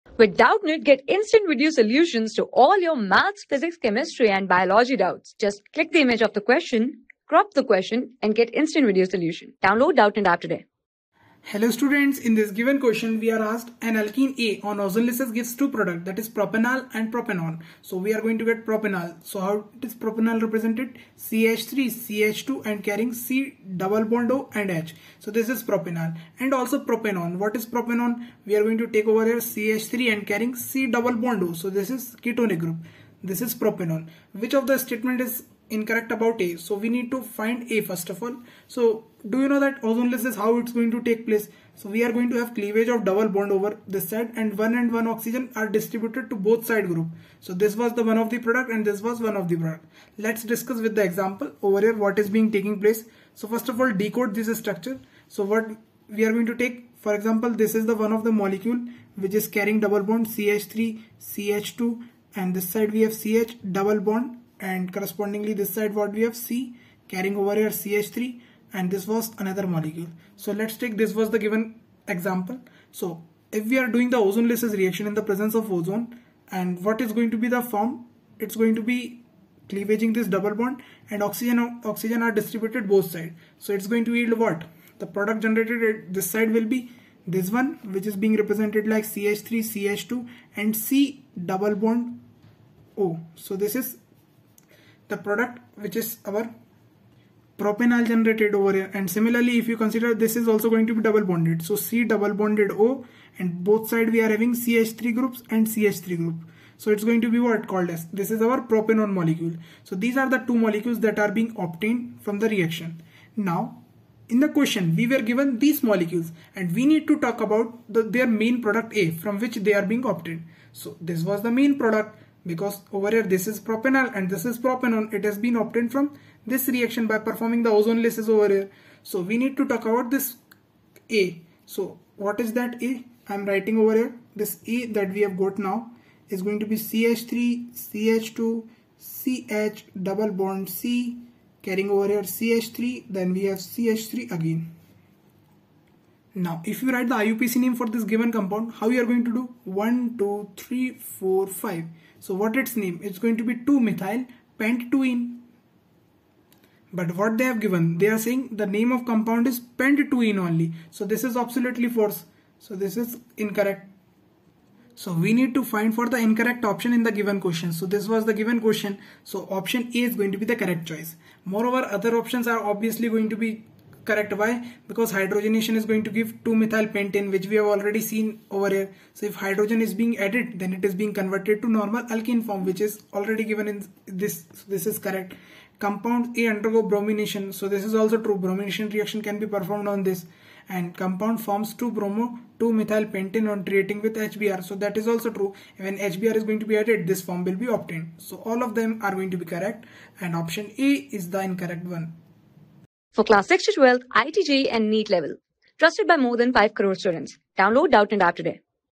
An alkane, A on ozonolysis gives two products propanal and propanone. Which of the given stateme...
An alkane, A on ozonolysis gives two products propanal and propanone. Which of the given statements is incorrect for A?
Class: 12
Subject: CHEMISTRY
Chapter: MOCK TEST 32
Board:IIT JEE

👉 Have Any Query? Ask Us.